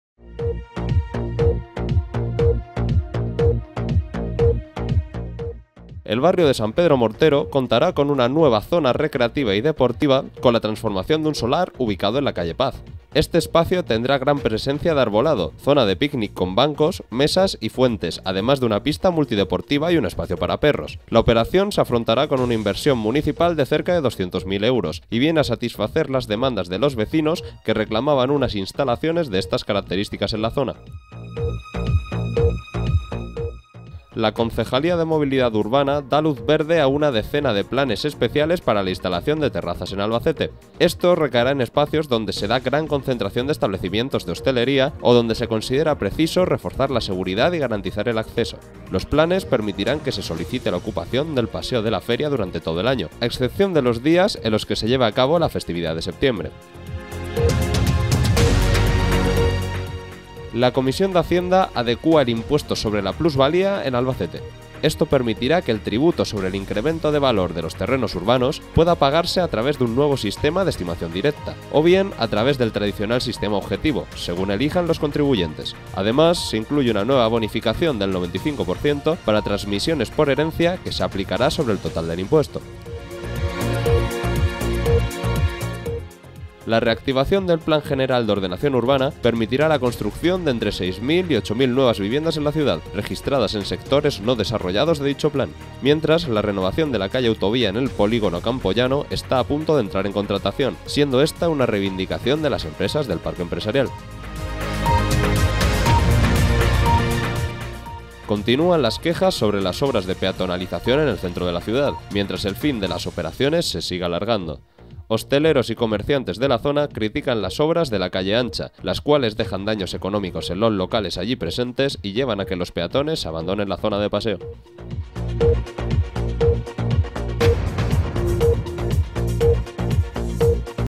Luz verde a los planes de terraza de una decena de establecimientos
-Nueva zona recreativa y deportiva en el barrio San Pedro Mortero
-Adecuación del impuesto de plusvalía en Albacete
-El POU del ayuntamiento permite la construcción de unas 8.000 viviendas
-Hosteleros y comerciantes, indignados con la duración de las obras